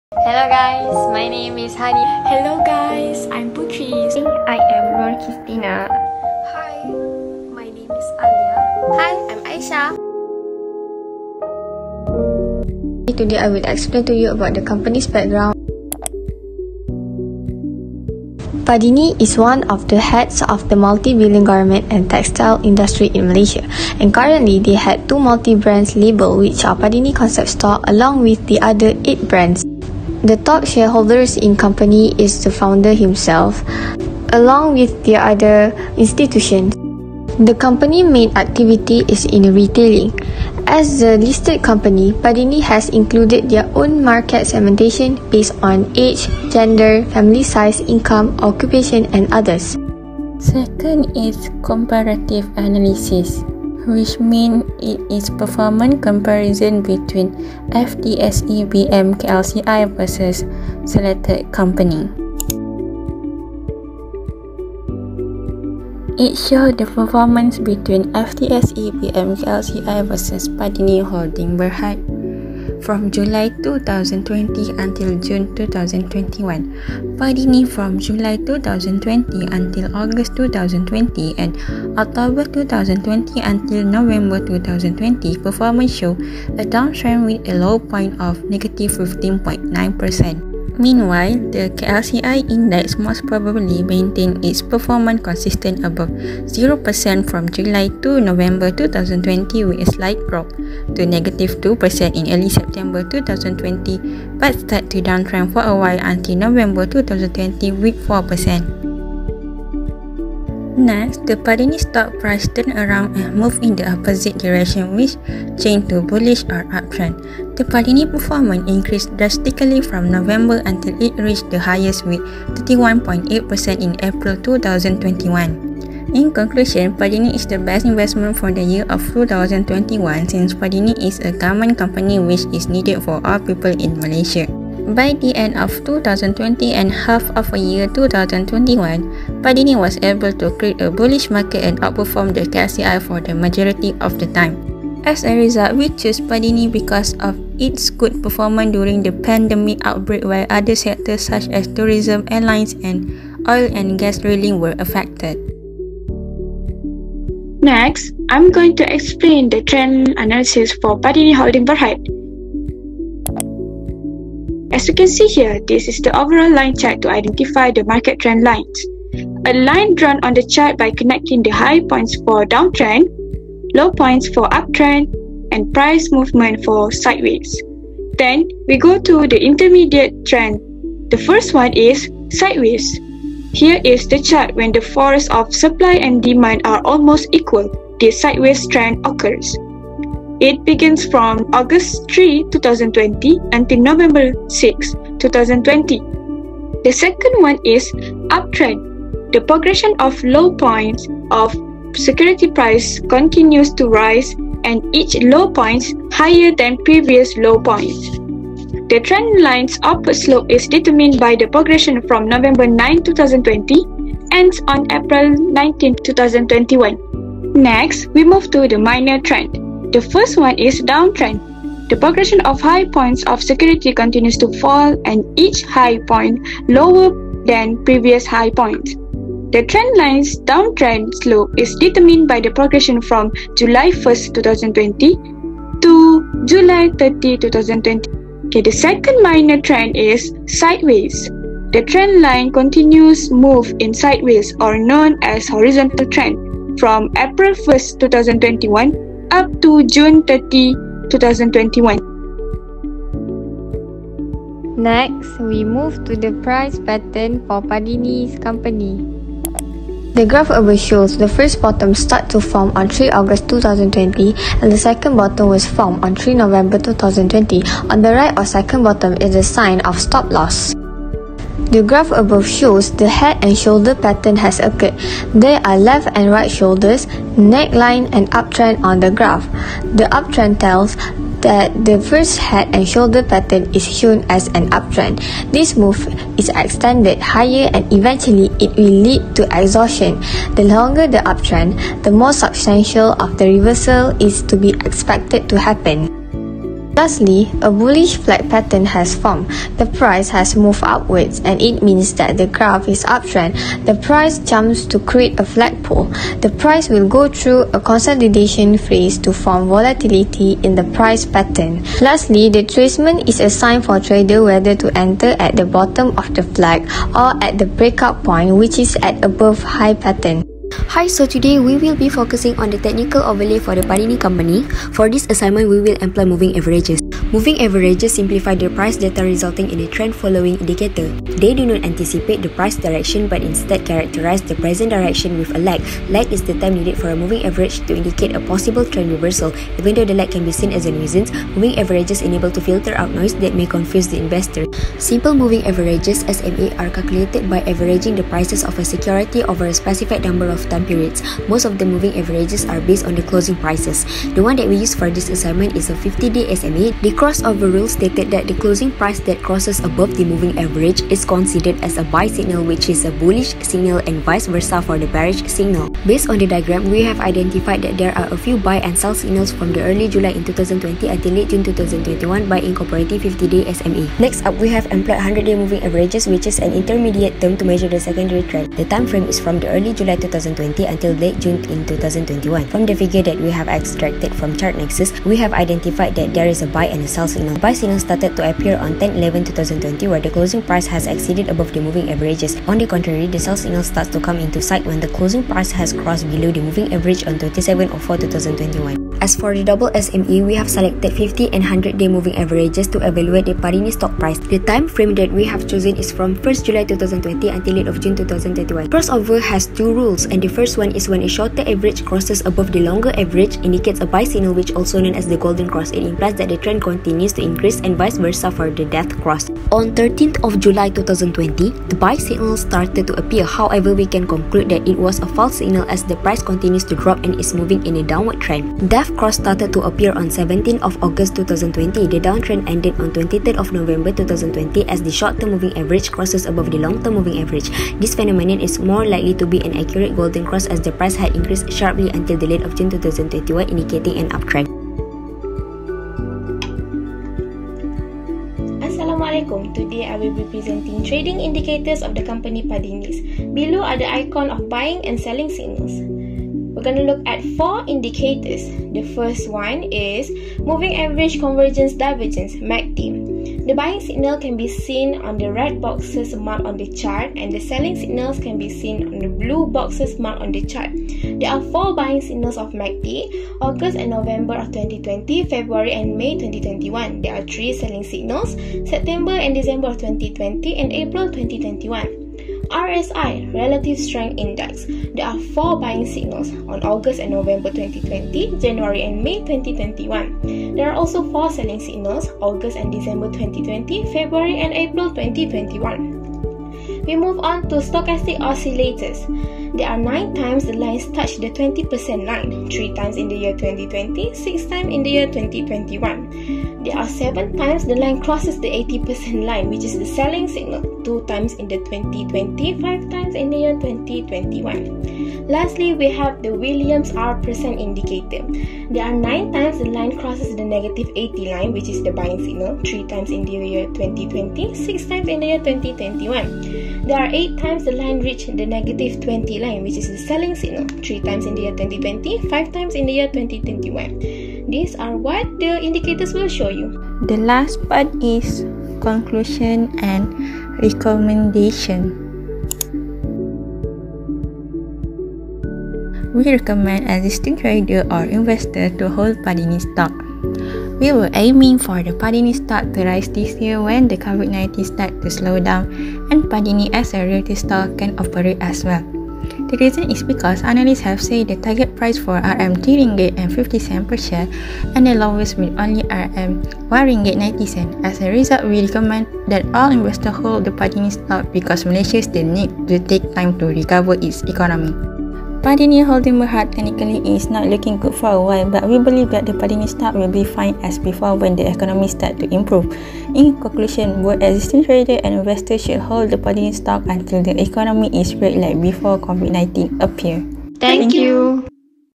FIN348 - Company Analysis (PADINI HOLDINGS BERHAD)
Video presentation for company analysis, UiTM Terengganu Kampus Dungun

Subject: Introduction of technical analysis (FIN348)
lecturer: Madam Suhaily Maizan Binti Abdul Manaf

Class: BA1144D

Group 2 : PADINI HOLDINGS BERHAD
Member details ;

1. Hanie Nabilah
2. Nur Aisyah Alya binti Ramzan
3. Nurqistina Aqilah binti Ramli
4. Aisyah Izzati binti Mohd Amrul Azri
5. Puteri Aina Natasha binti Nizman